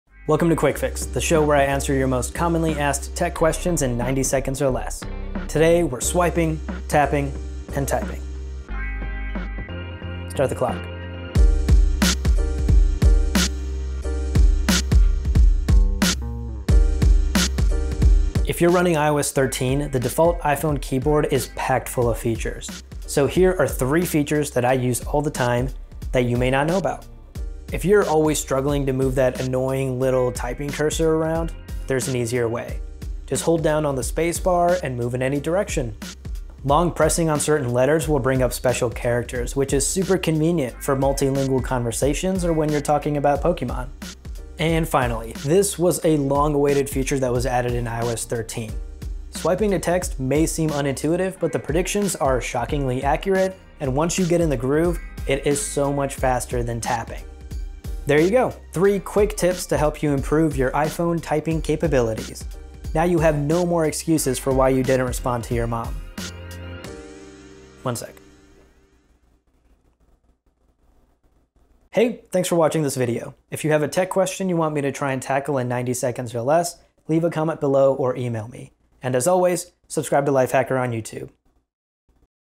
The iOS Keyboard Shortcuts You Should Know About
The default iOS keyboard in iOS 13 is full of useful features that you might have missed. In the video above, I share three features I use all the time.

Easily move the typing cursor

There is nothing more annoying than trying to precisely move the tiny typing cursor with your finger. Luckily, there’s an easier way: Just long-press on the spacebar and you’ll see the letter keys fade away. While keeping your finger pressed down, simply move it in any direction to easily control that pesky cursor.

Special characters

Long-pressing on certain characters will bring up a submenu of special characters like “é”. This is especially helpful in multilingual conversations and long-winded arguments about Pokémon. (Fun fact: This trick also works in the desktop OS.)